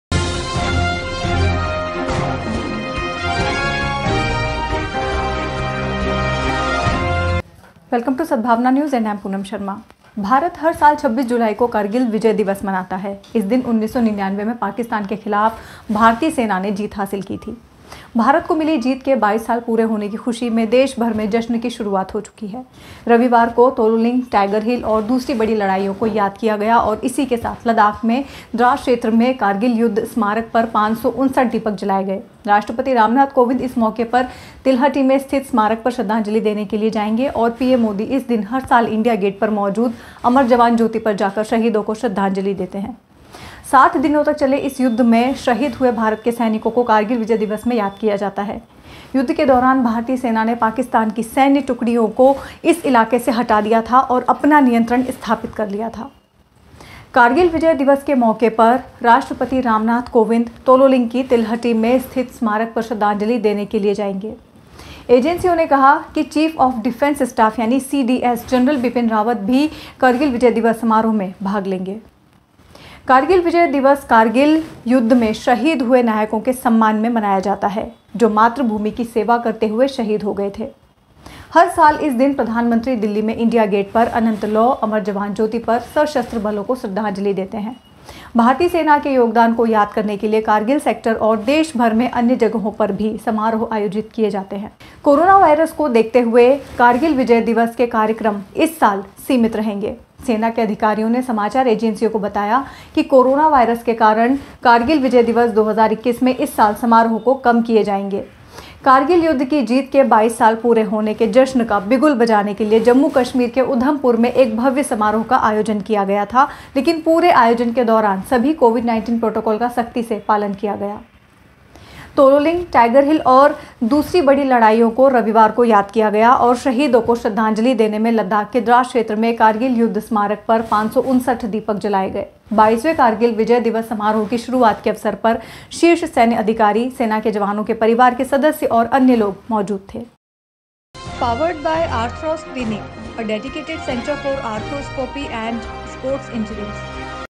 National News - करगिल विजय दिवस 2021 - शहीदों का किया जाएगा सम्मान, लद्दाख में जलाए गए 559 दीपक
भारत हर साल 26 जुलाई को 'करगिल विजय दिवस' मनाता है। इस दिन 1999 में पाकिस्तान के खिलाफ भारतीय सेना ने जीत हासिल की थी। भारत को मिली जीत के 22 साल पूरे होने की खुशी में देशभर में जश्न की शुरुआत हो चुकी है। रविवार को तोलोलिंग, टाइगर हिल और दूसरी बड़ी लड़ाईयों को याद किया गया और इसी के साथ लद्दाख में द्रास क्षेत्र में करगिल युद्ध स्मारक पर 559 दीपक जलाए गए। राष्ट्रपति रामनाथ कोविंद इस मौके पर तिलहटी में स्थित स्मारक पर श्रद्धांजलि देने के लिए जाएंगे और पीएम मोदी इस दिन हर साल इंडिया गेट पर मौजूद अमर जवान ज्योति पर जाकर शहीदों को श्रद्धांजलि देते हैं।

60 दिनों तक चले इस युद्ध में शहीद हुए भारत के सैनिकों को 'करगिल विजय दिवस' में याद किया जाता है। युद्ध के दौरान भारतीय सेना ने पाकिस्तान की सैन्य टुकड़ियों को इस इलाके से हटा दिया था और अपना नियंत्रण स्थापित कर लिया था।

'करगिल विजय दिवस' के मौके पर राष्ट्रपति रामनाथ कोविंद तोलोलिंग की तिलहटी में स्थित स्मारक पर श्रद्धांजलि देने के लिए जाएंगे।  एजेंसियों ने कहा कि चीफ ऑफ डिफेंस स्टाफ (सीडीएस) जनरल बिपिन रावत भी क'रगिल विजय दिवस' समारोह में भाग लेंगे।

'करगिल विजय दिवस' कारगिल युद्ध  में शहीद हुए नायकों के सम्मान में मनाया जाता है, जो मातृ भूमि की सेवा करते हुए शहीद हो गए थे। हर साल इस दिन, प्रधान मंत्री दिल्ली में इंडिया गेट पर 'अनन्त लौ', अमर जवान ज्योति पर सशस्त्र बलों को श्रद्धांजलि देते हैं। भारतीय सेना के योगदान को याद करने के लिए करगिल सेक्टर और देश भर में अन्य जगहों पर भी समारोह आयोजित किए जाते हैं।

कोरोना वायरस को देखते हुए 'करगिल विजय दिवस' के कार्यक्रम इस साल सीमित रहेंगे।  सेना के अधिकारियों ने समाचार एजेंसियों को बताया कि कोरोना वायरस के कारण 'करगिल विजय दिवस 2021' में  इस साल समारोह को कम किए जाएंगे। करगिल युद्ध की जीत के 22 साल पूरे होने के जश्न का बिगुल बजाने के लिए जम्मू-कश्मीर के उधमपुर में एक भव्य समारोह का आयोजन किया गया था, लेकिन पूरे आयोजन के दौरान सभी कोविड -19 प्रोटोकॉल का सख्ती से पालन किया गया।

तोलोलिंग, टाइगर हिल और दूसरी बड़ी लड़ाईयों को रविवार को याद किया गया और शहीदों को श्रद्धांजलि में लद्दाख के द्रास क्षेत्र में कारगिल युद्ध स्मारक पर 559 दीपक जलाए गए। 22वें करगिल विजय दिवस समारोह की शुरुआत के अवसर पर शीर्ष सैन्य अधिकारी, सेना के जवानों के परिवार के सदस्य और अन्य लोग मौजूद थे।